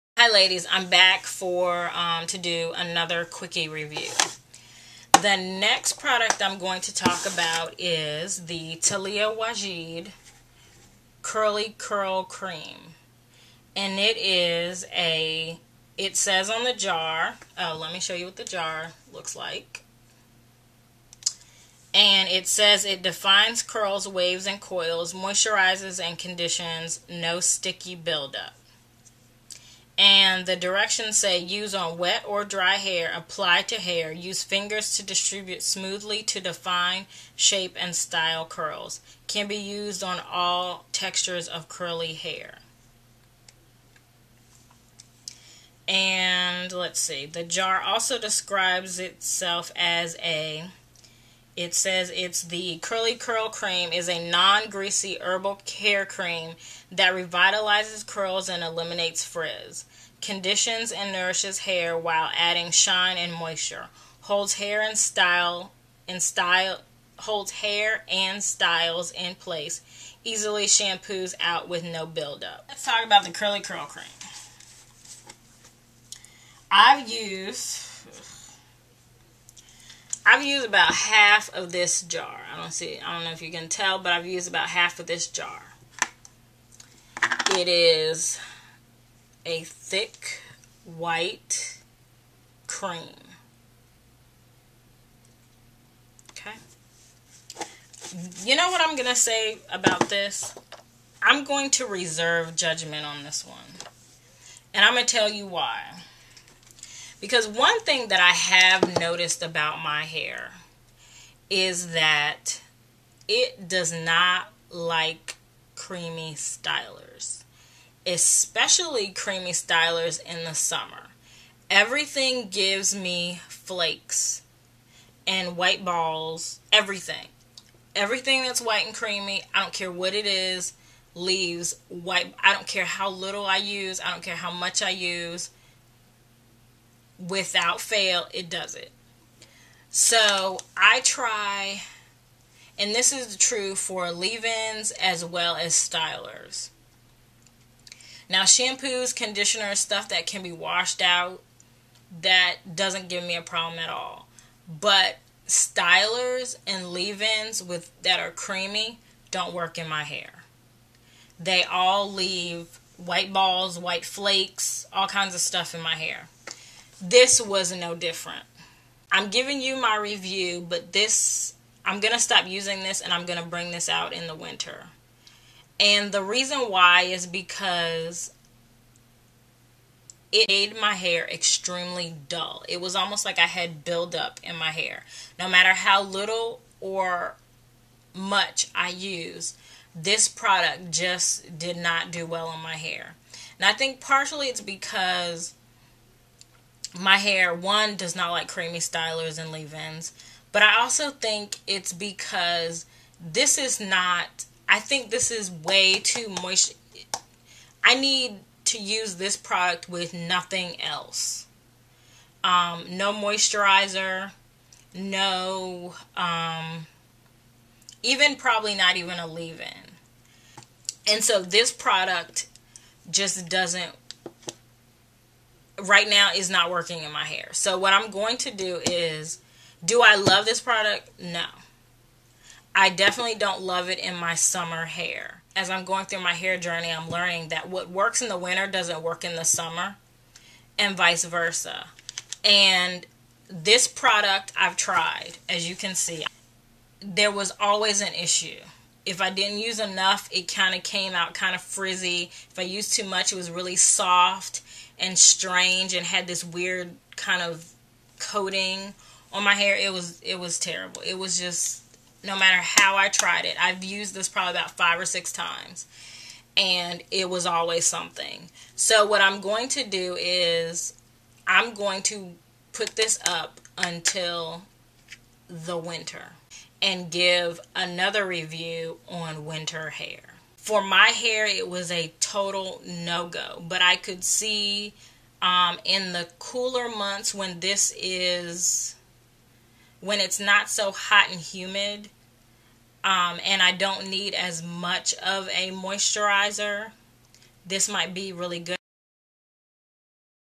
Review: Taliah Waajid Curly Curl Cream
This is a quick review on the Taliah Waajid Curly Curl Cream.  I will revisit this product again in the cooler months.